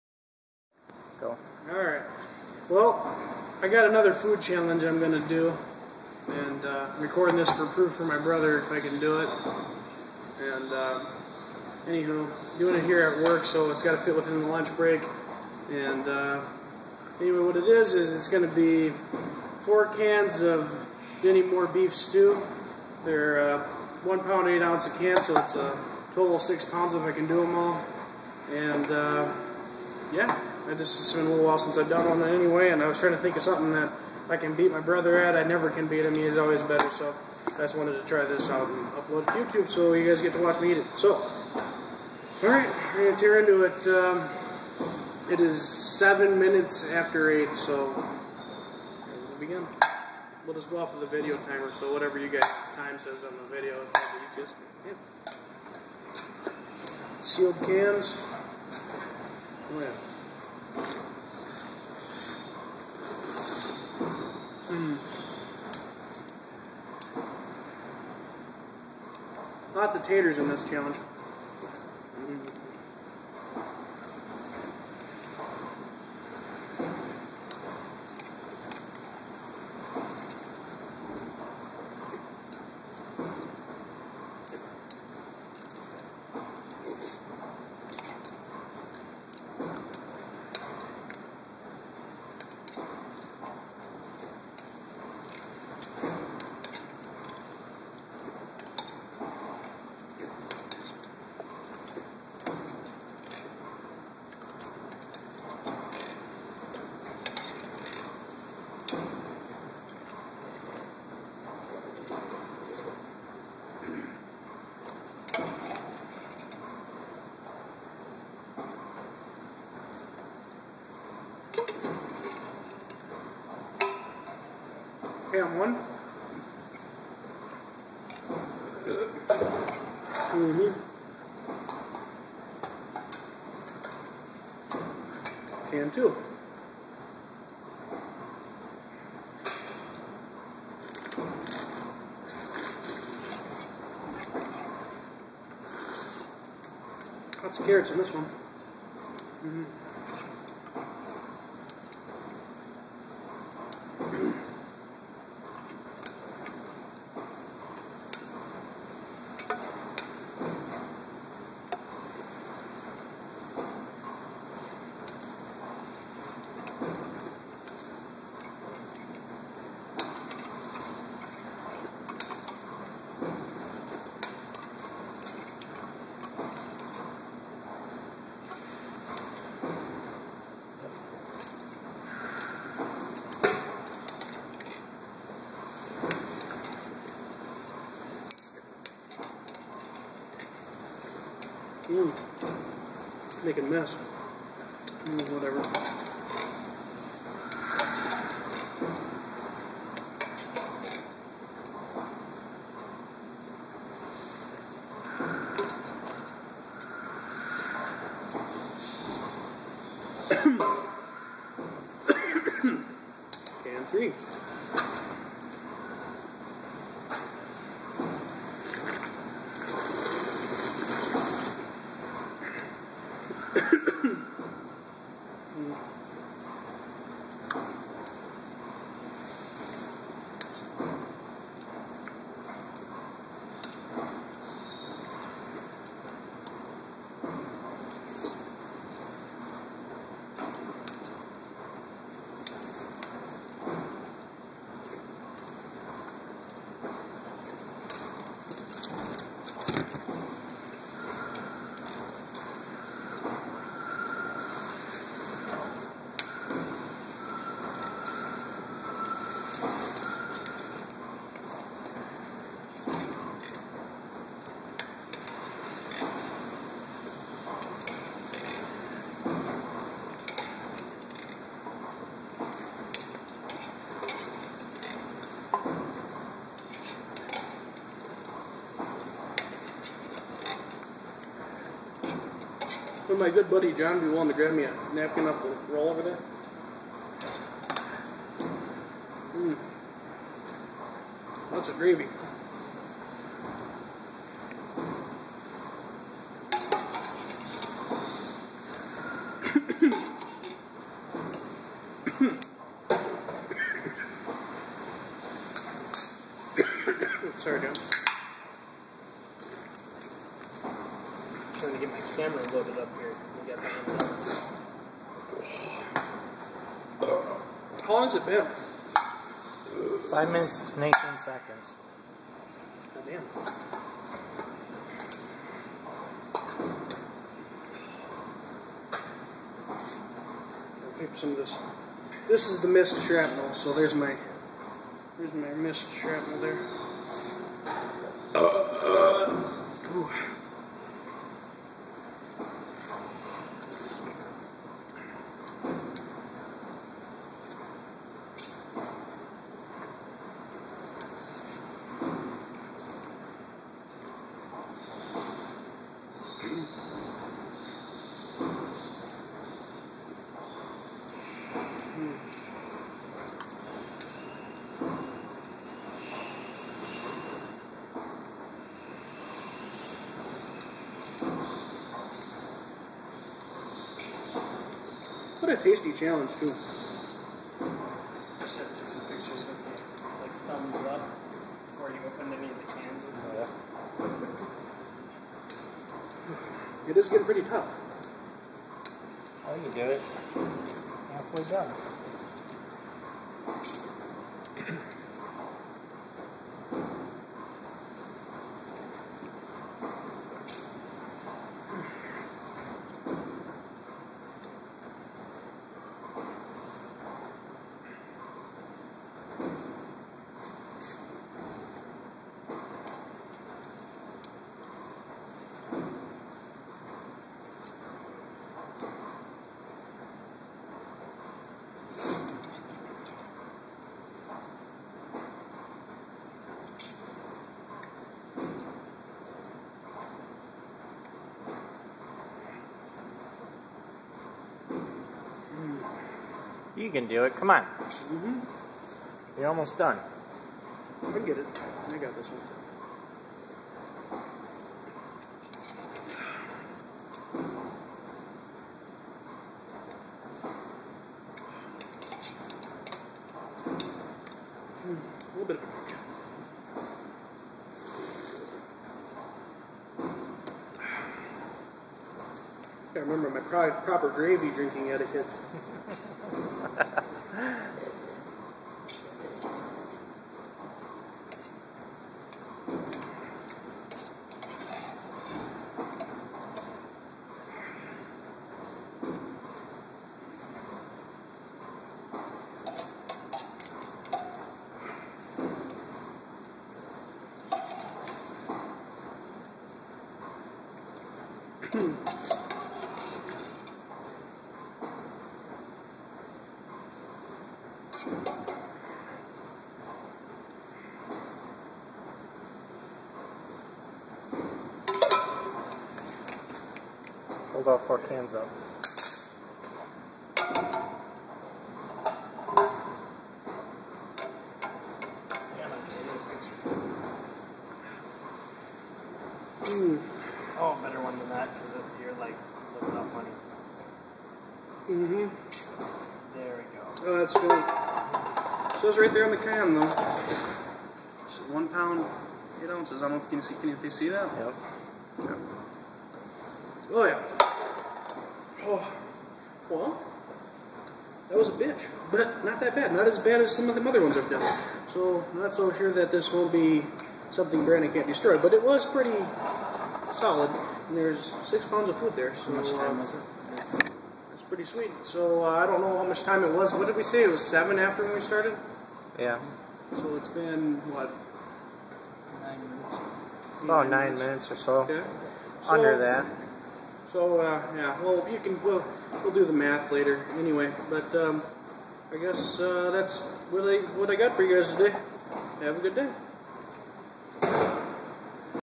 6 lbs of stew challenge just over 8 minutes
I ate 6 pounds of stew in just over 8 minutes with a bit of mtn dew, my brother tied me but did not really beat me on this one thanks for watching guys